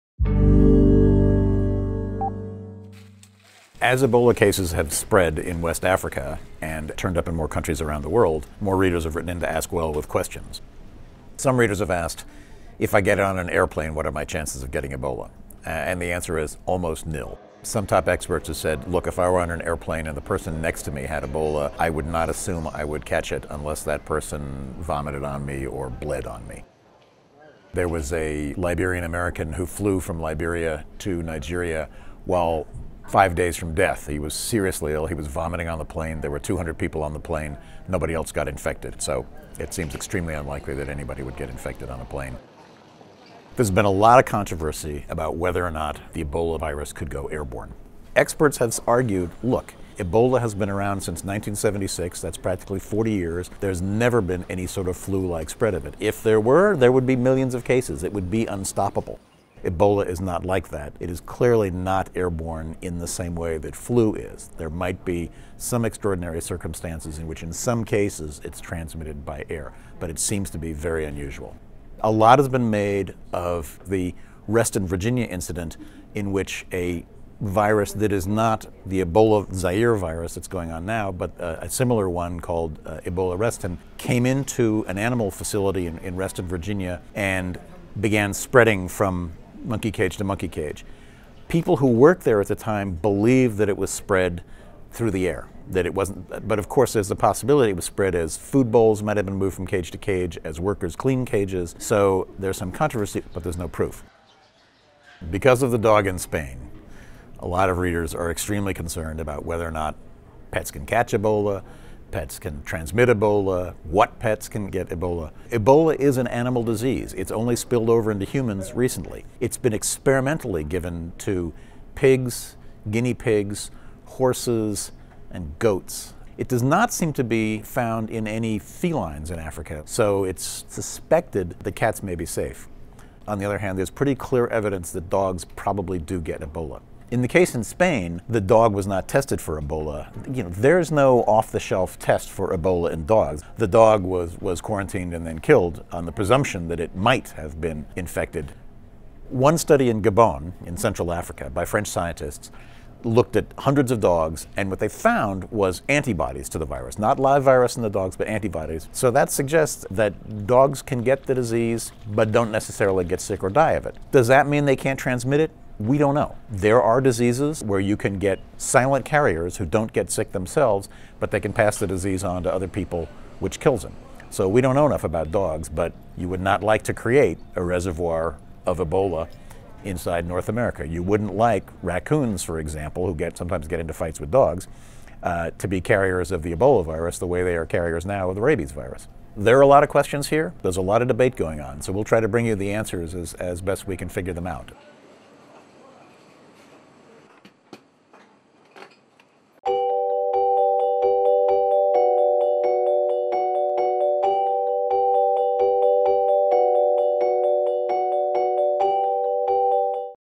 Ebola Virus Outbreak 2014: Can Pets Spread It? | Ask Well | The New York Times
Donald G. McNeil Jr. answers reader questions about the Ebola virus.

Produced by: Colin Archdeacon